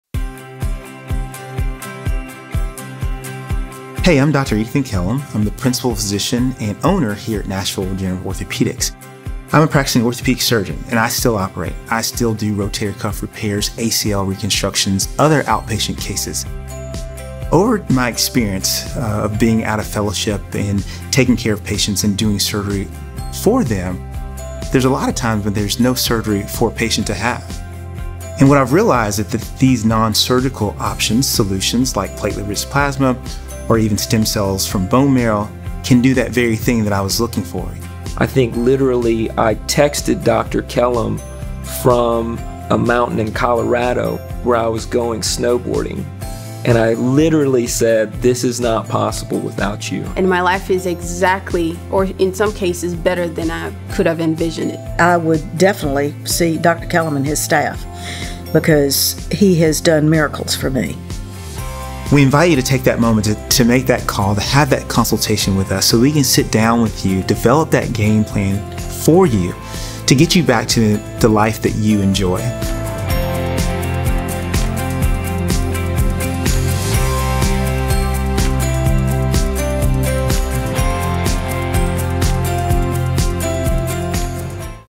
Getting You Back to Living the Life You Want | Nashville [2021]
Whether you require a lifestyle change, diet changes, physical therapy, stem cells, PRP or surgery, we will work out a plan to return you to a pain-free life.

Additionally, there is amounting evidence showing that stem cell and platelet rich plasma (PRP) therapies significantly aid in the recovery process after surgery.

For a consultation on how to best resolve your knee arthritis or osteoarthritis, please give our office a call at .

Here’s to your health and longevity!